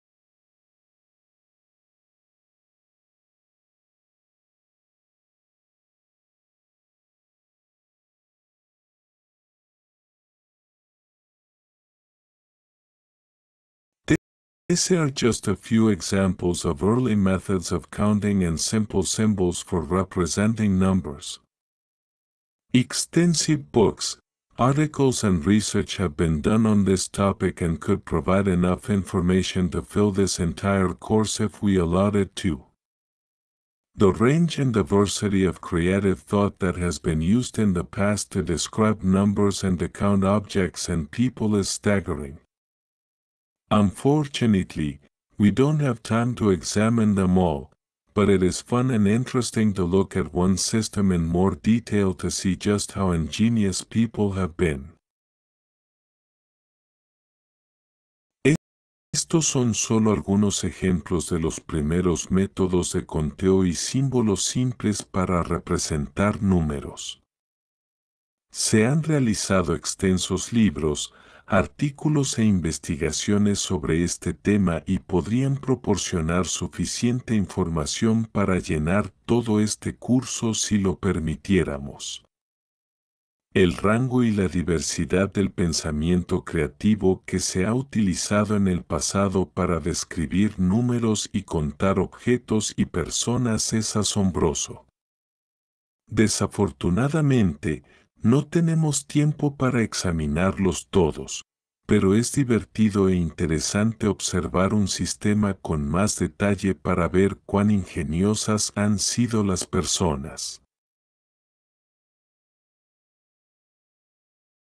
Using Edge Split Screen with Luis and ReadAloud TTS for English-Spanish Translation
Using Microsoft Edge in "Split Screen" mode with "Microsoft Luis Online (Natural) - Spanish (Ecuador)" ReadAloud TTS Voice to read a math homework passage in English and then in translated Spanish.
Even though the Spanish is too advanced for me yet, I'm attuning my ears to it.